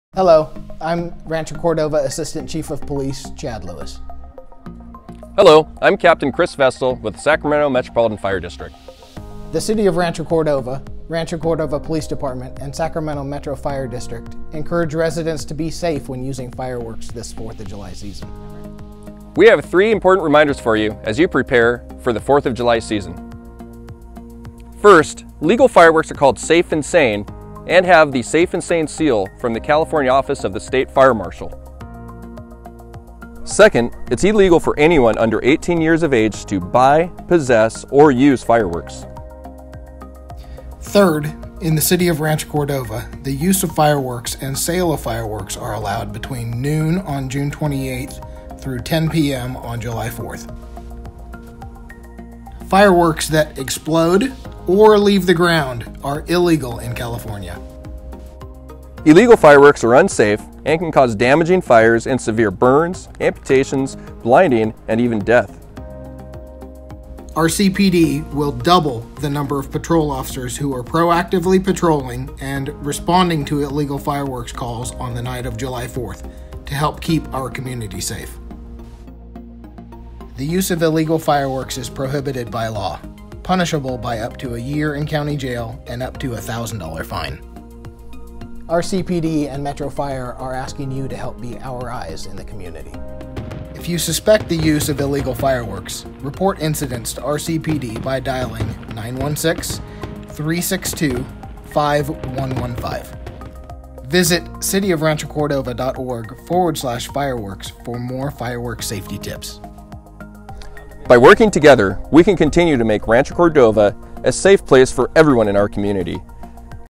2019 Fireworks Safety
The City of Rancho Cordova, Rancho Cordova Police Department and Sacramento Metro Fire
District encourage residents to be safe when using fireworks this Fourth of July season.